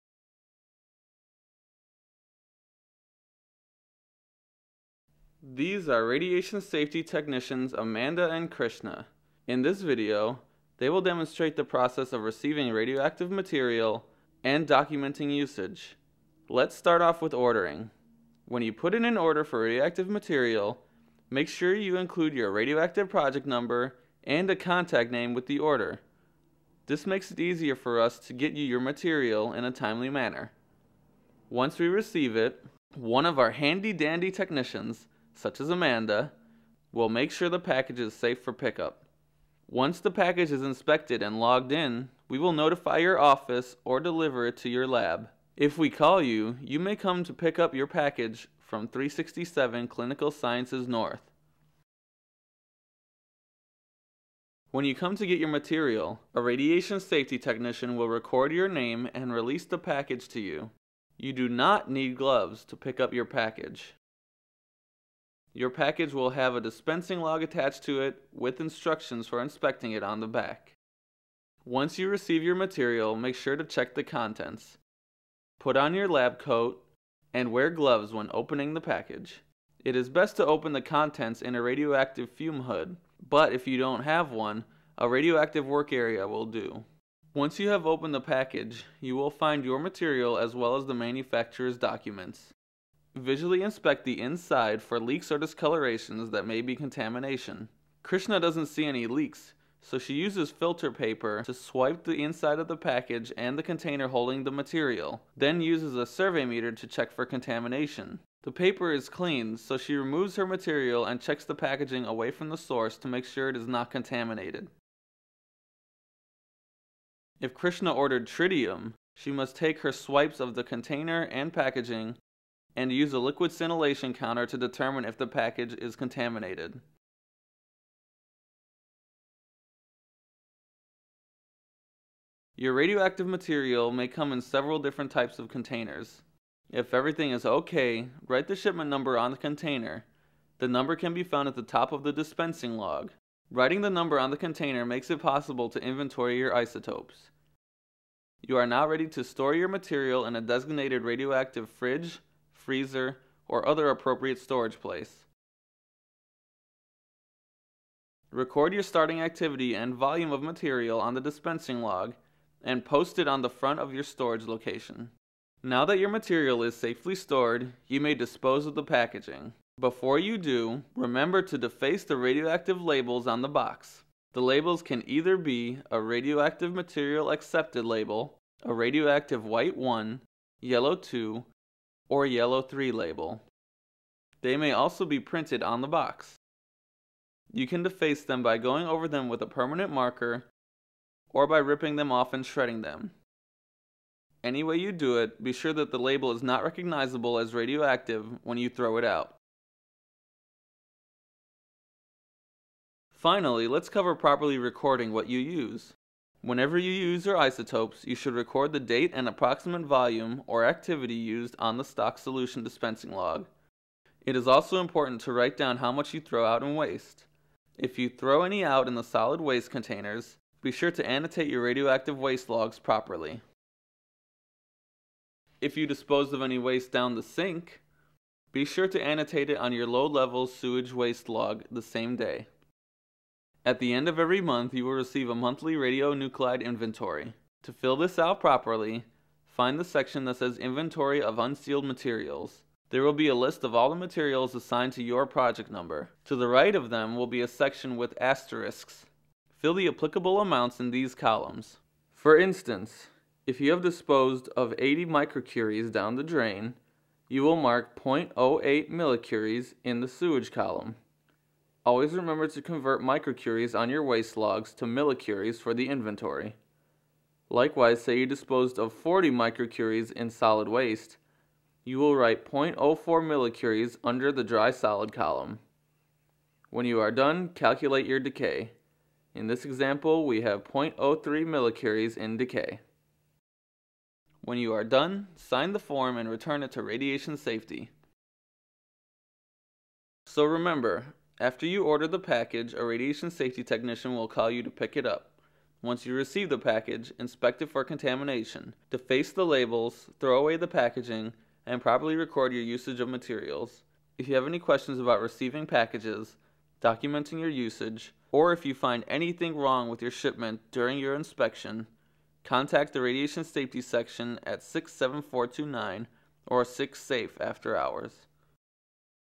Radioactive Shipment procedures for laboratories
How to receive radioactive packages.  Made by Eric Galicia for UIC's Radiation Safety Section.